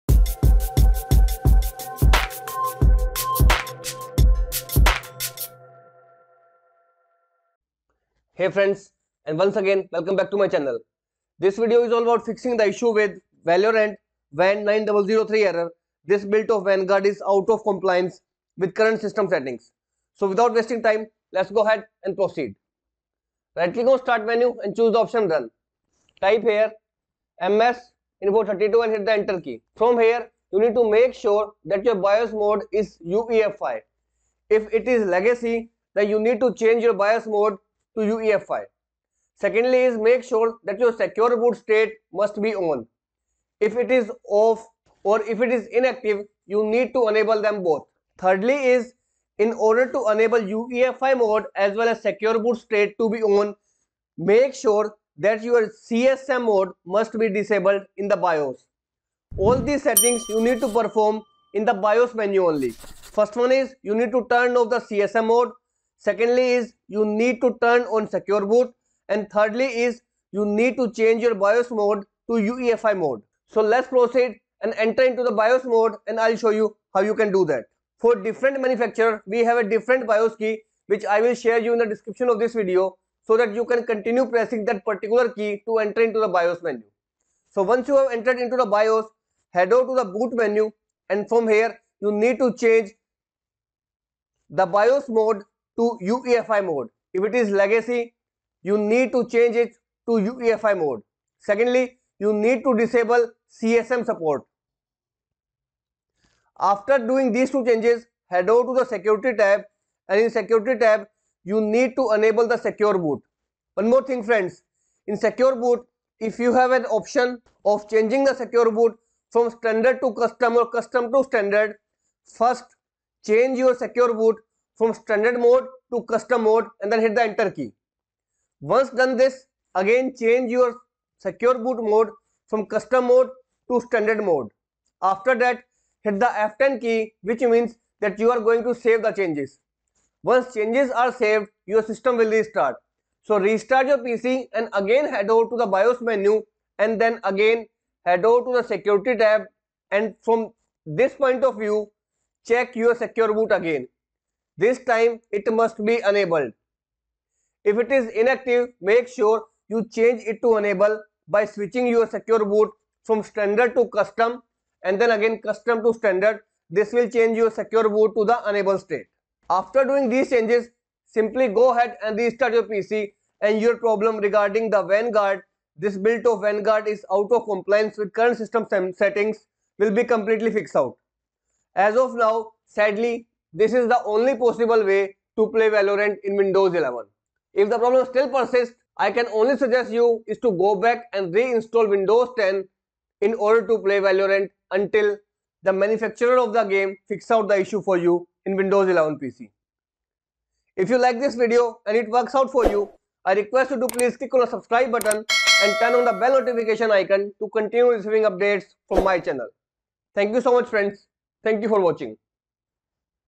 Valorant VAN 9003- This build of vanguard is out of compliance with current system settings
This video is all about fixing the valorant van 9003 error this build of vanguard is out of compliance with current system settings with error code VAN 9003 as with the windows 11 microsoft has come up with the more security and so does the valorant. Valorant has imposed the requirement of secure boot as well as tpm 2.0 to play valorant which is causing the issue while launching the riot vanguard and you got the error of out of compliance with van 9003 error.

BIOS key for different manufacturer :

Here's a list of common BIOS keys by brand. Depending on the age of your model, the key may be different.
ASRock: F2 or DEL
ASUS: F2 for all PCs, F2 or DEL for Motherboards
Acer: F2 or DEL
Dell: F2 or F12
ECS: DEL
Gigabyte / Aorus: F2 or DEL
HP: F10
Lenovo (Consumer Laptops): F2 or Fn + F2
Lenovo (Desktops): F1
Lenovo (ThinkPads): Enter then F1.
MSI: DEL for motherboards and PCs
Microsoft Surface Tablets: Press and hold volume up button.
Origin PC: F2
Samsung: F2
Toshiba: F2
Zotac: DEL